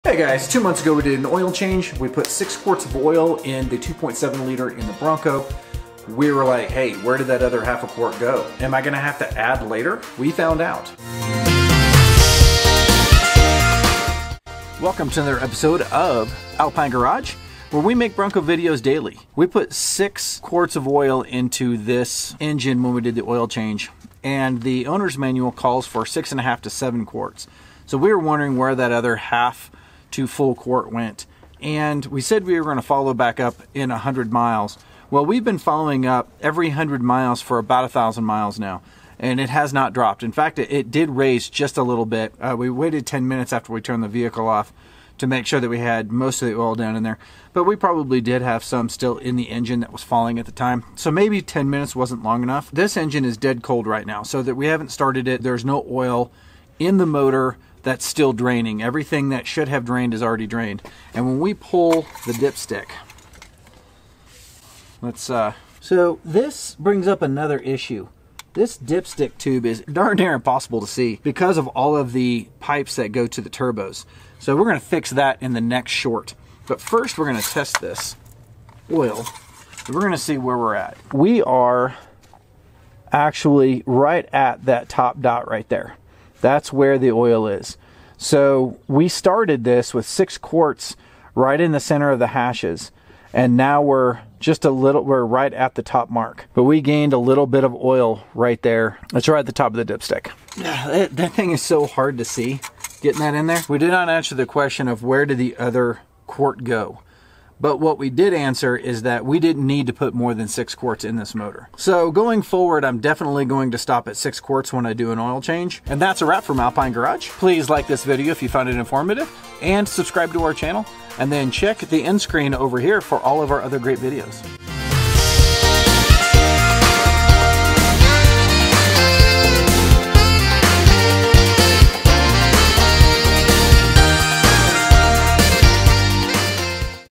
Ford Bronco - Where's The Other Quart? We Added 6 Quarts, Did We Add Again?
In our Ford Bronco 2.7 ecoboost oil change video, we found out that initially you only need 6 qts.  We checked back every 100 miles until we hit an additional 1000 miles.  What did we find?  Unbelievable, it actually gained oil level!  It may be that the dip stick is wrong and does not show the correct oil level, and we may be running a bit lean.  Only time will tell.

Official Ford Bronco Store for Tee's and Accessories

Tictok: mile.3garage